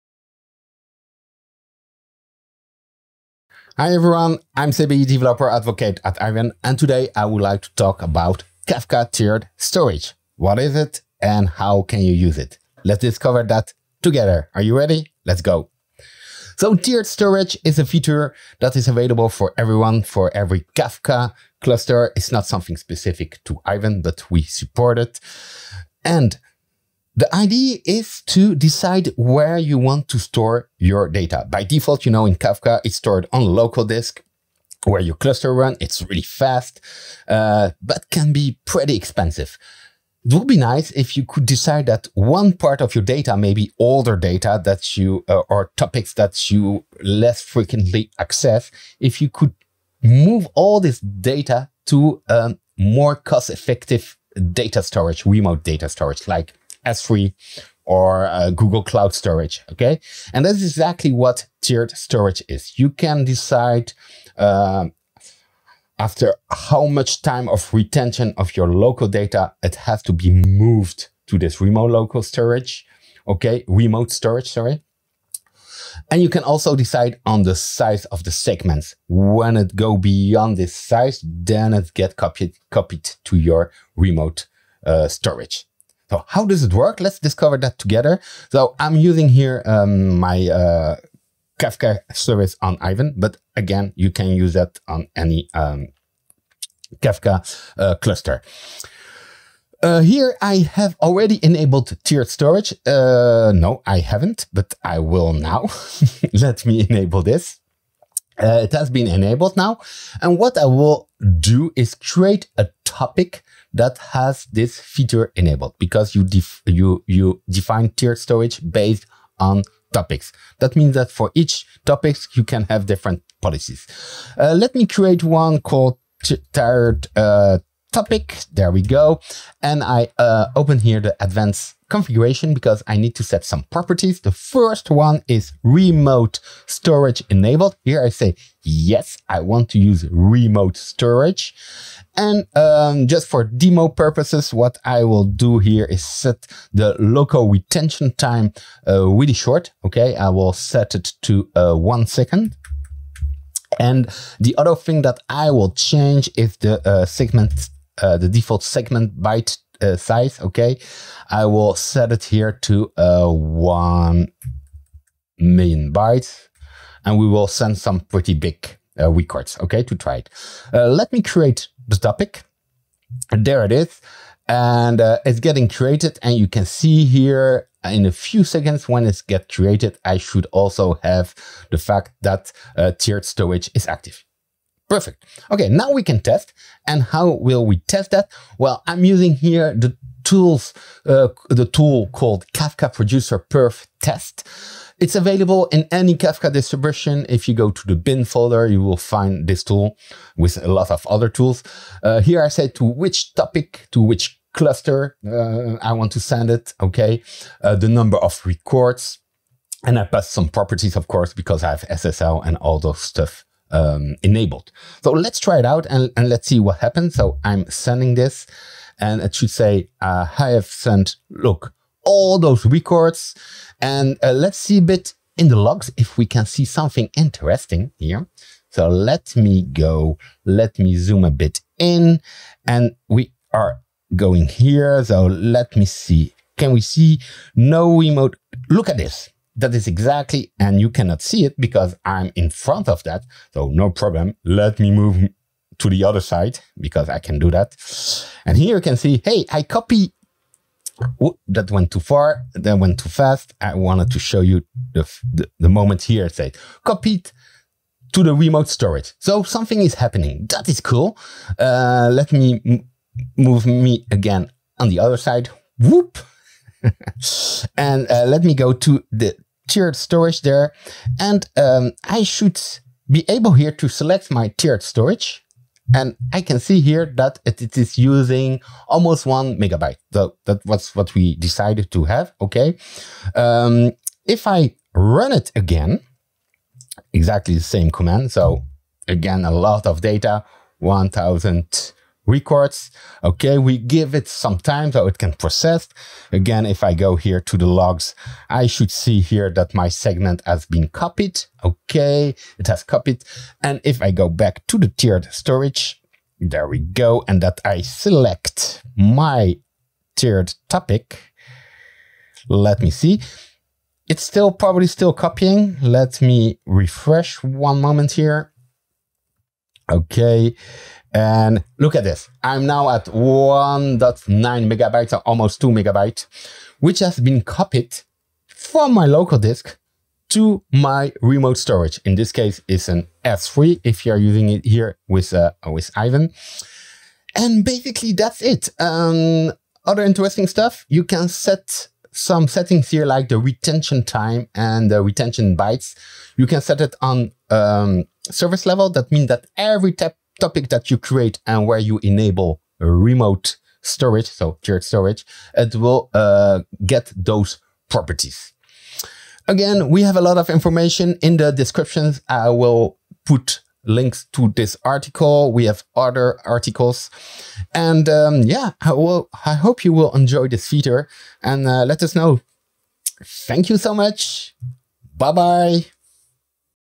Explore Apache Kafka® Tiered Storage
Get an introduction to  Apache Kafka® Tiered Storage which enables more effective data management by utilizing two different storage types—local disk and remote cloud storage solutions such as Amazon S3, Google Cloud Storage, and Azure Blob Storage. This feature offers a tailored approach to data storage, allowing you to allocate frequently accessed data to high-speed local disks while offloading less critical or infrequently accessed data to more cost-effective remote storage solutions. Tiered storage enables you to indefinitely store data on specific topics without running out of space. Once enabled, it is configured per topic, giving you granular control over data storage needs.

(links to any tools or resources you used in the video, our docs/trial if appropriate)